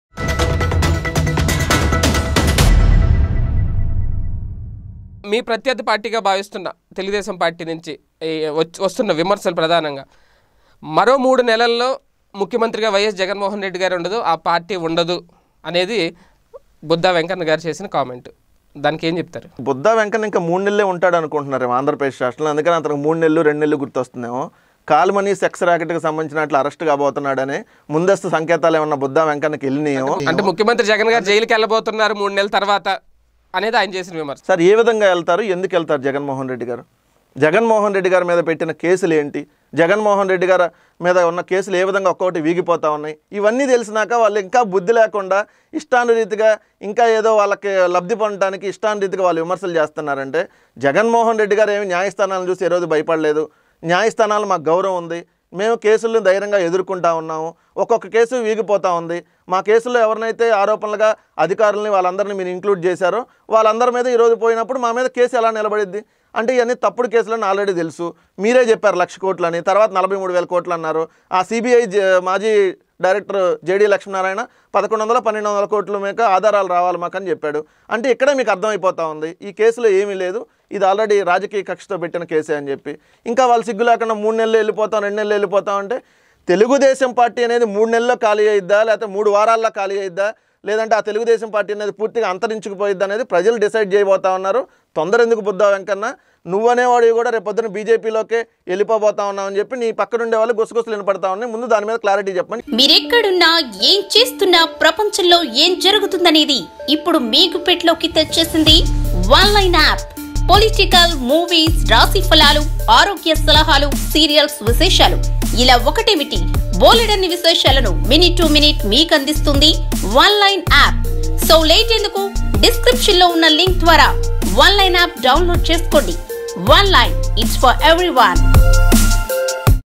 Karumuri Venkata Reddy Strong Counter to Buddha Venkanna | AP CM Jagan | PDTV NEWS
బుద్దా వెంకన్నకు  గూబ పగిలేలా సమాధానం ఇచ్చిన వెంకట్ రెడ్డి